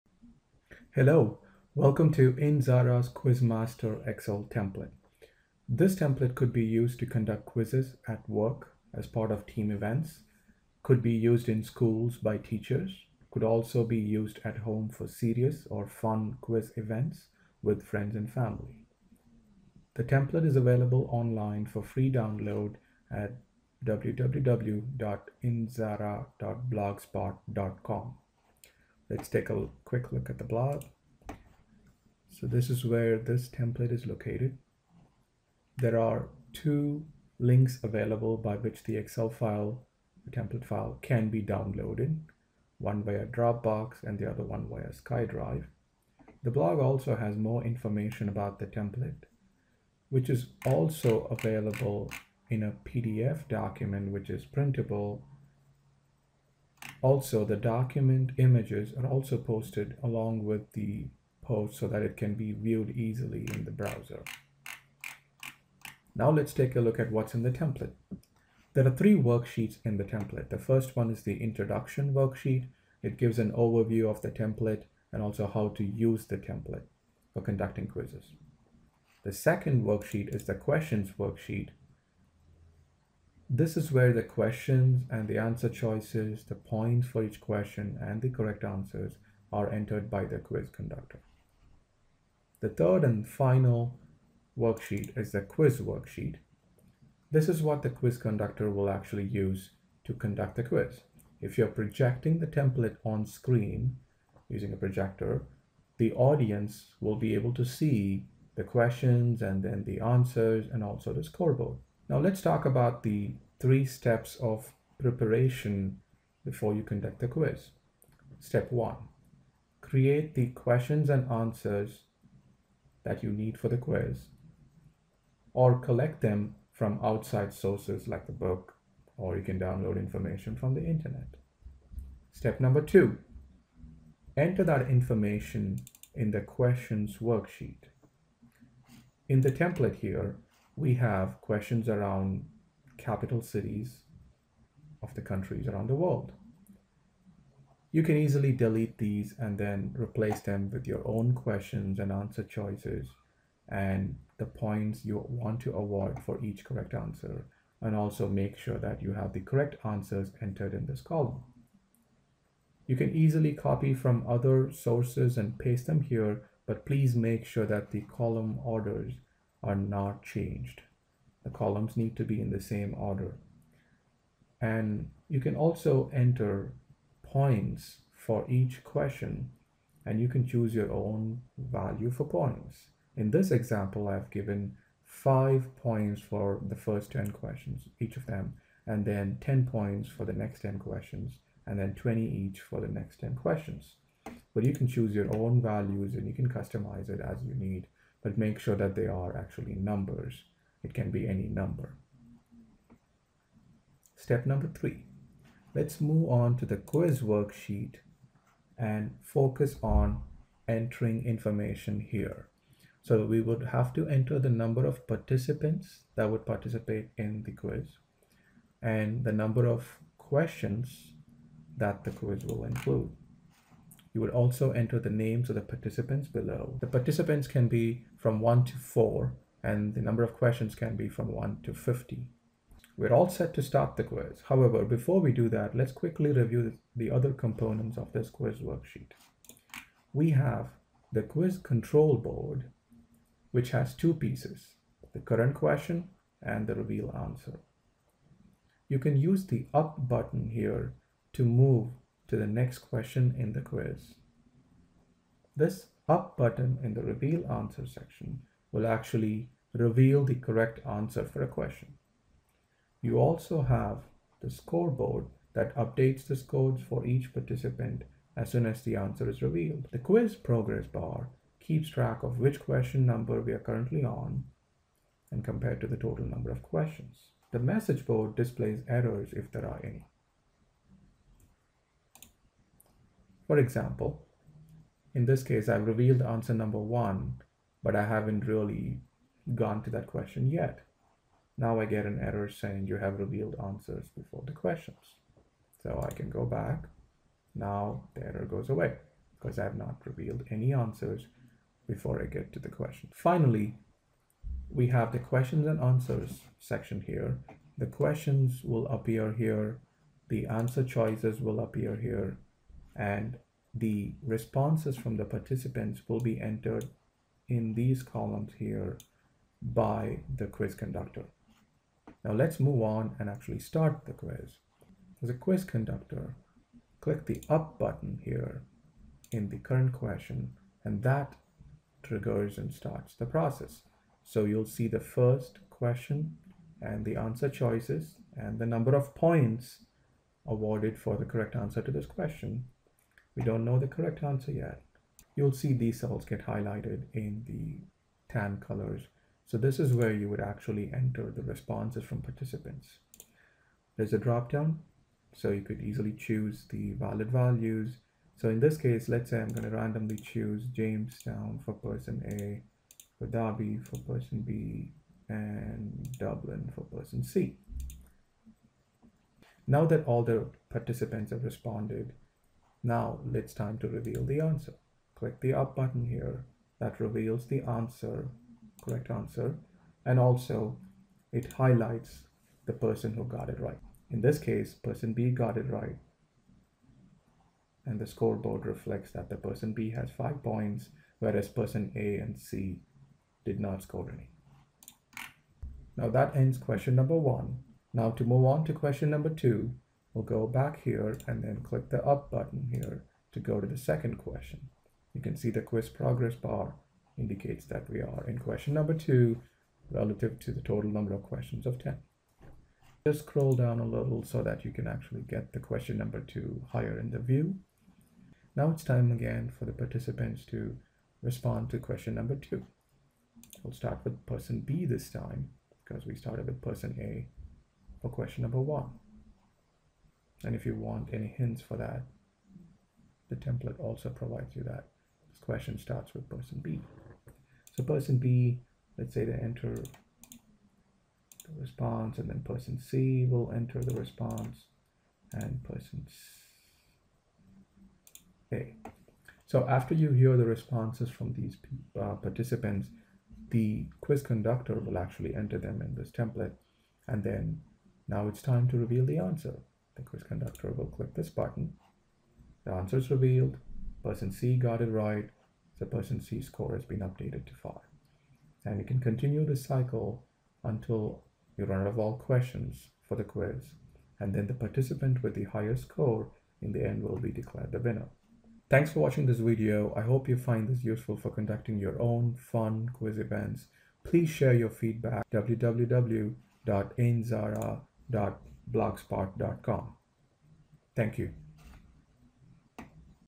Quiz Master Excel Template by indzara
Quiz Master Excel Template by indzara; This can be used to conduct fun quizzes in team events; Teachers can use this to quiz students in schools; The template can be downloaded